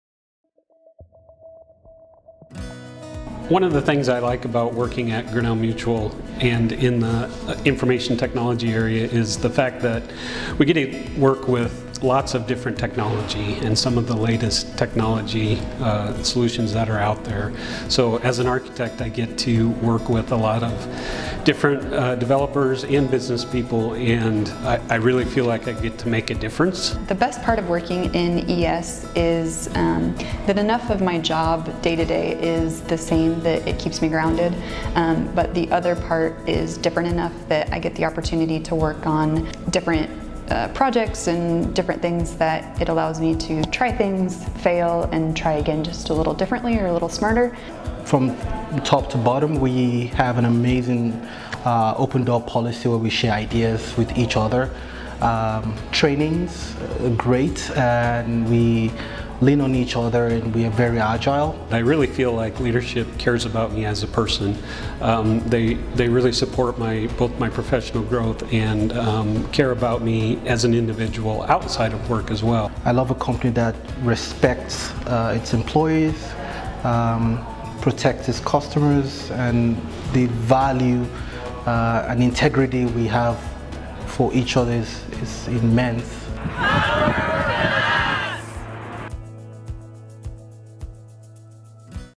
IT jobs at Grinnell Mutual
Learn about our IT (ES) department and why people love working there.